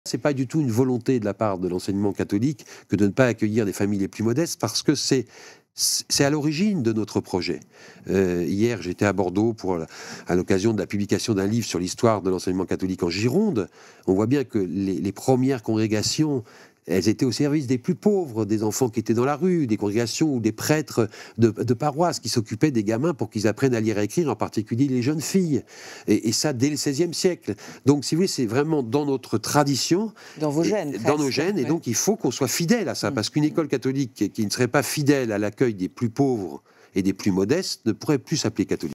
Philippe Delorme rappelle que «l’accueil des plus modestes, c’est dans nos gènes»
Invité de «Points de Vue», Philippe Delorme (@EnsCatho) rappelle que «l’accueil des plus modestes, c’est dans nos gènes. Une école catholique qui ne serait pas fidèle à l’accueil des plus pauvres ne pourrait plus s’appeler catholique».